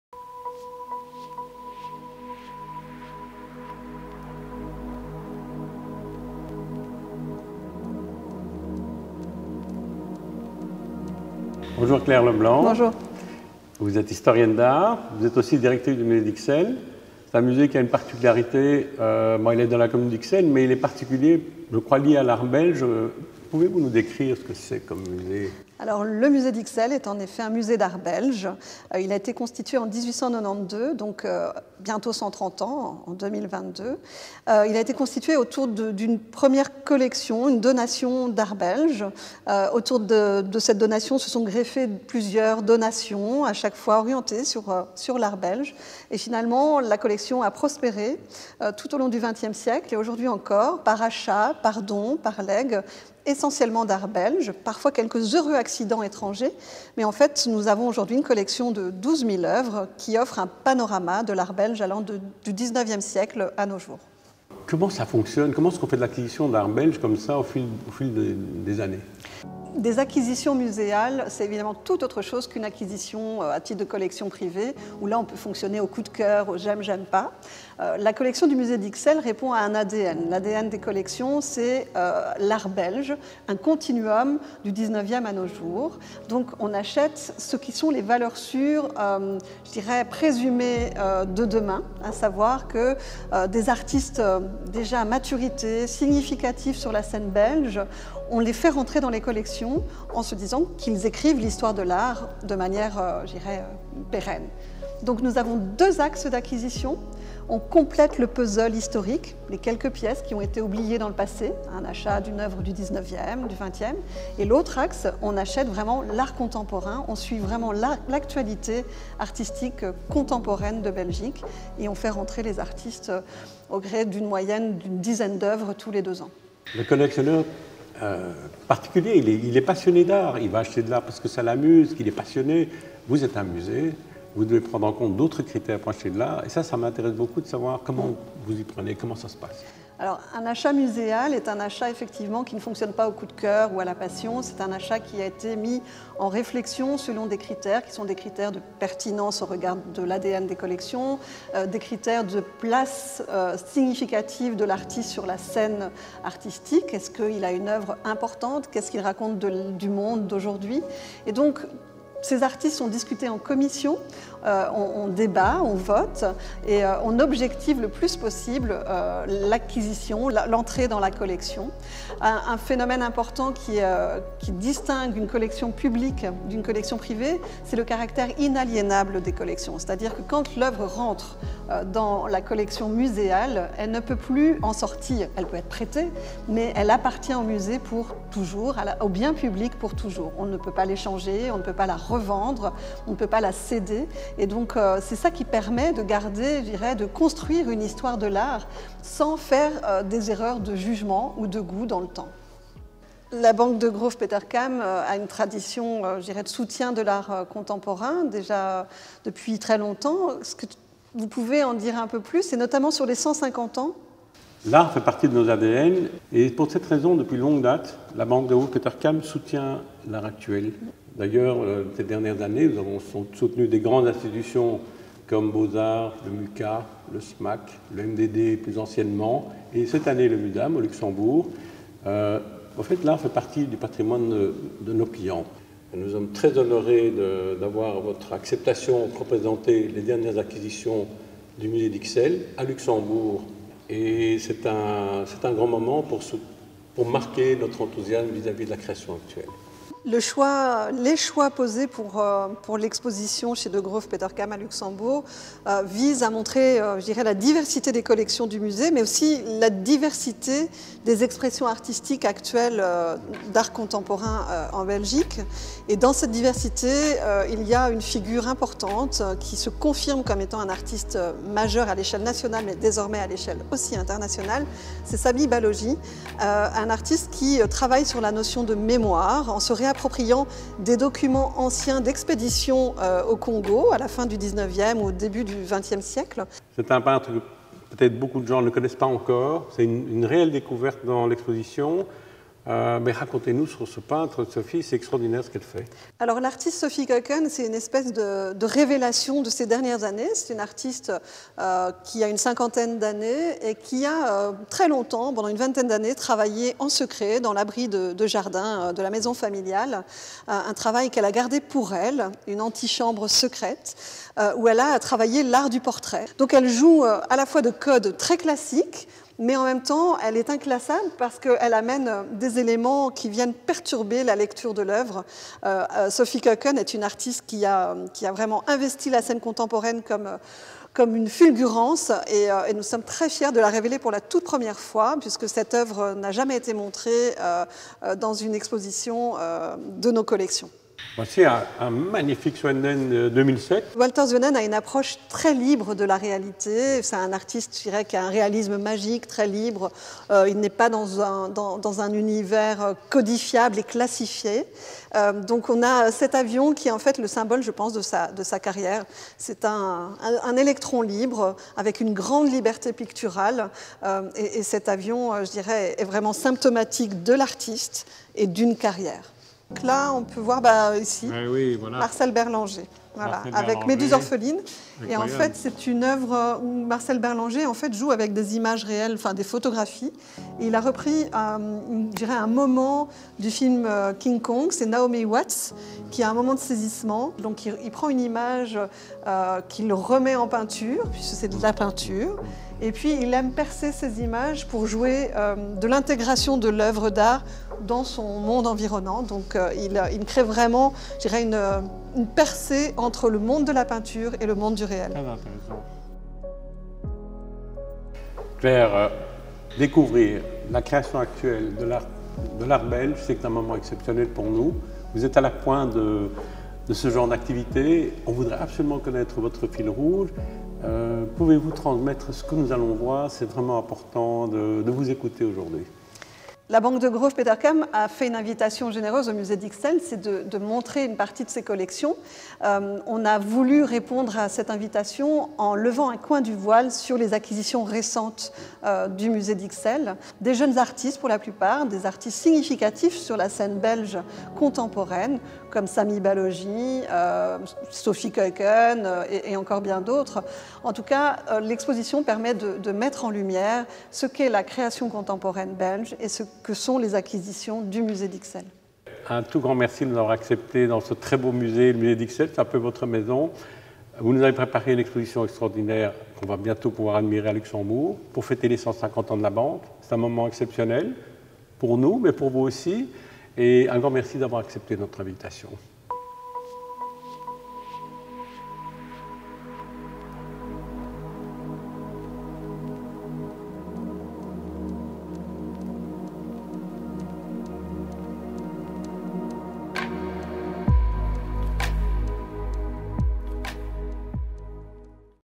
"Close up on Belgian contemporary art"
Exposition "Close up on Belgian contemporary art"

Montée en partenariat avec le Musée d’Ixelles à Bruxelles, cette exposition est dédiée à l’art contemporain belge et coïncide avec les 150 ans de Degroof Petercam. Une sélection d’œuvres acquises ces 30 dernières années par le Musée sera présentée, comme les créations de Marcel Berlanger, Sophie Kuijken, Stephan Balleux, Sammy Baloji et Anne De Gelas. Certaines œuvres seront d’ailleurs exposées au public pour la première fois.

Filmé à Bruxelles en juin 2021.

"Close up on Belgian contemporary art" Exhibition

A selection of artworks acquired by the Museum over the past 30 years will be presented, including works by Marcel Berlanger, Sophie Kuijken, Stephan Balleux, Sammy Baloji and Anne De Gelas. Some of the pieces will be on public display for the first time.

Filmed in Brussels in June 2021.